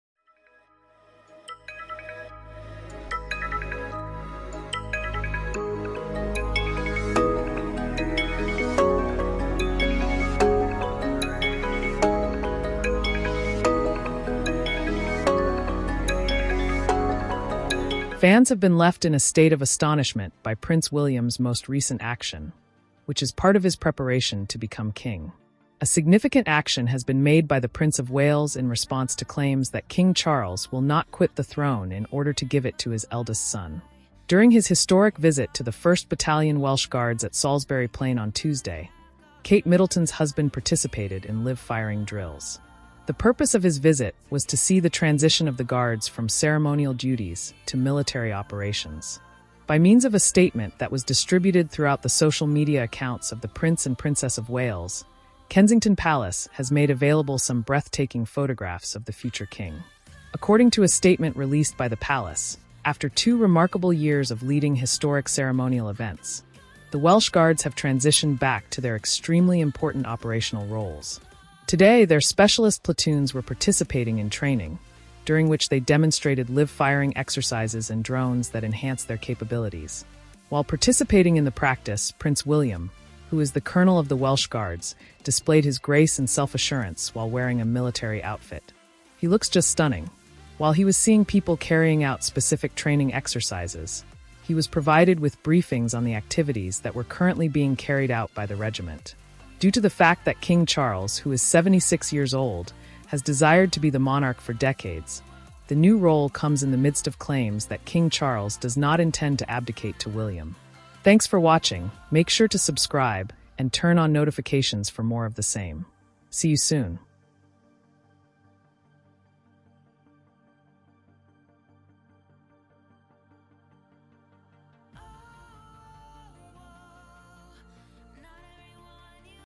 FED UP Prince William Takes DRASTIC Measures After King Charles Refusal To ABDICATE The Throne.
King Charles resistance on descending the throne makes prince William takes actions into his hands as he takes drastic measures